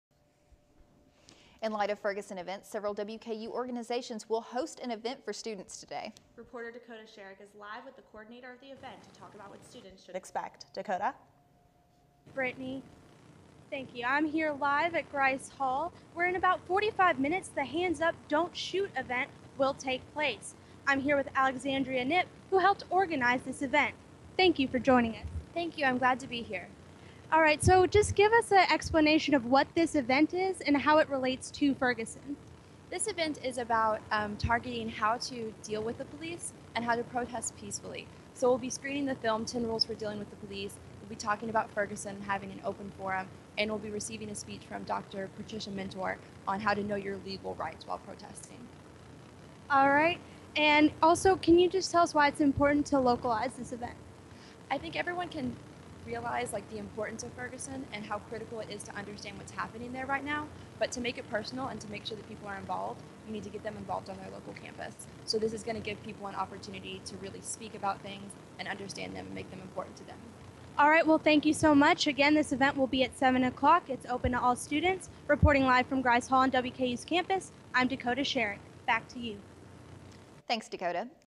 News Channel 12 Live Shot
I went live from Grise Hall on WKU's campus for News Channel 12 on December 2, 2014.